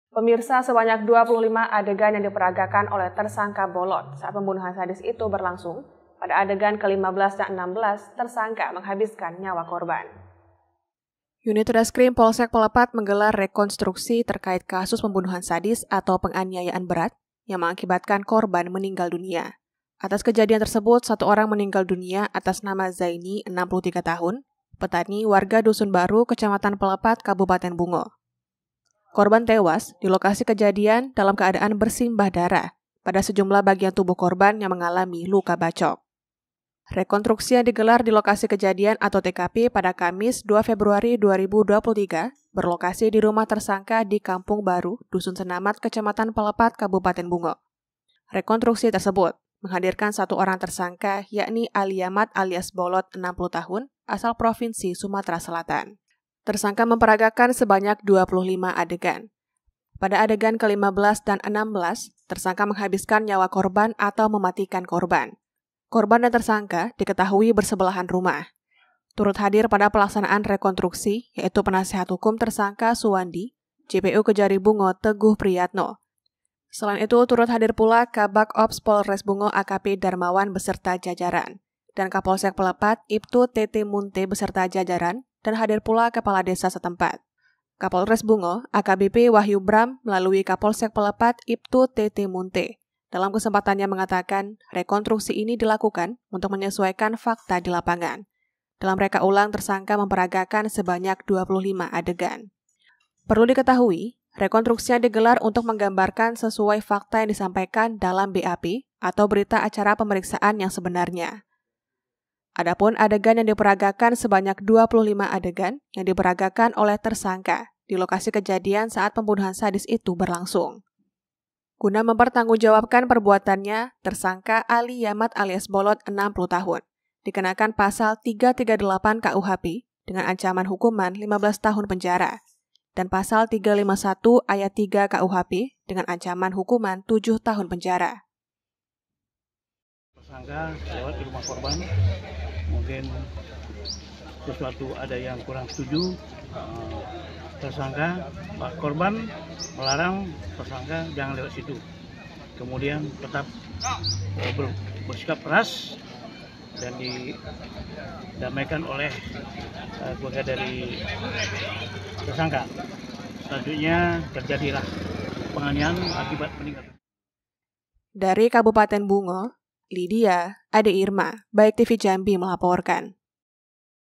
UNIT RESKRIM POLSEK PELEPAT GELAR REKONTRUKSI KASUS PEMBUNUHAN SADIS
BNEWS EDISI 3 MARET 2023